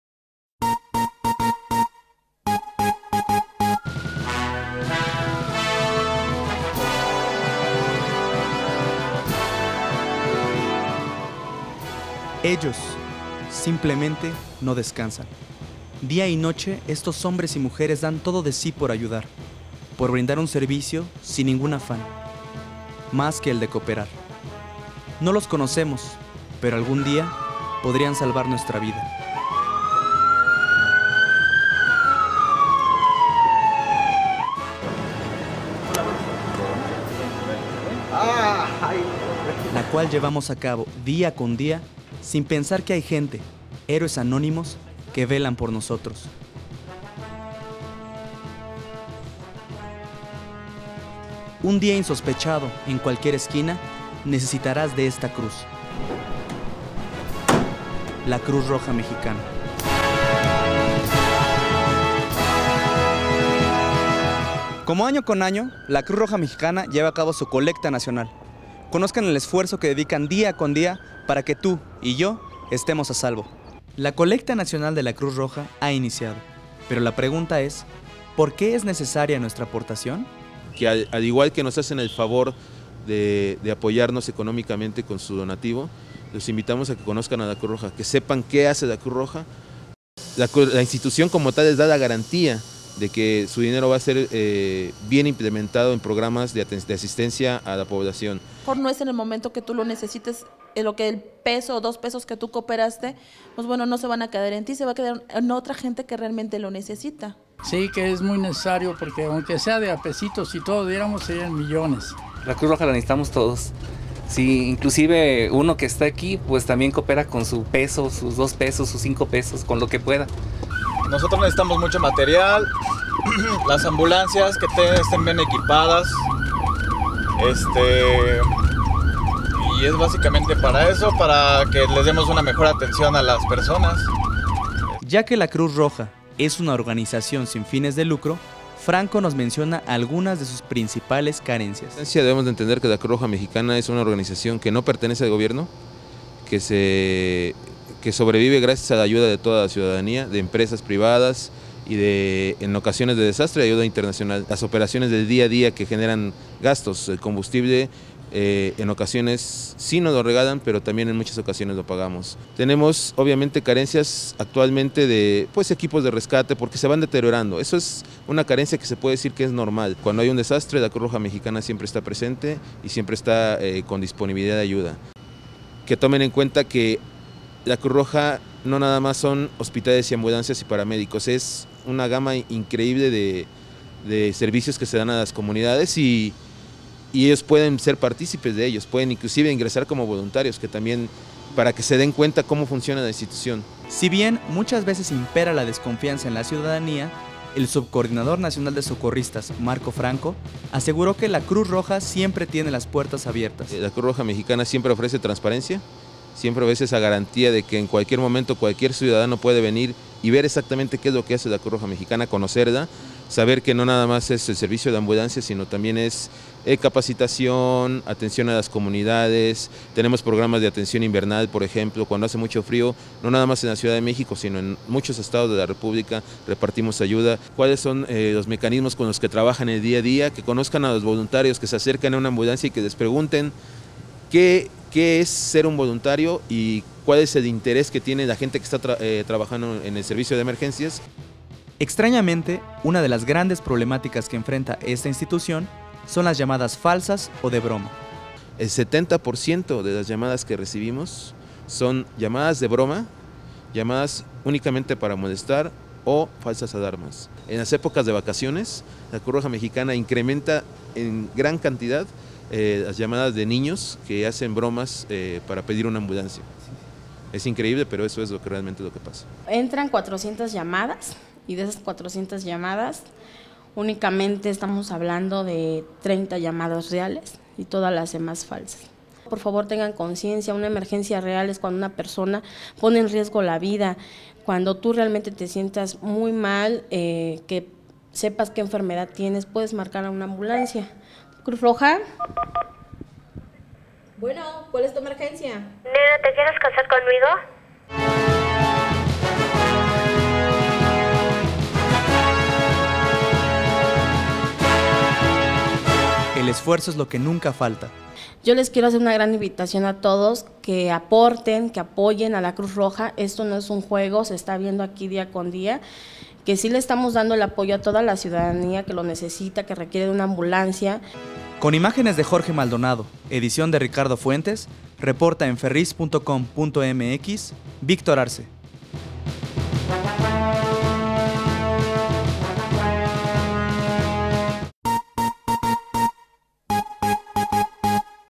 Cruz Roja: Ahora estos héroes te necesitan
Como cada año la Cruz Roja realiza su colecta anual para recaudar fondos, pues esta institución, que ayuda a la población en caso de desastres, accidentes o cualquier otra emergencia, se sostiene mayoritariamente a través de donativos.

Aquí te presentamos este reportaje sobre la importante labor de estos héroes anónimos que algún día cualquiera de nosotros podría necesitar.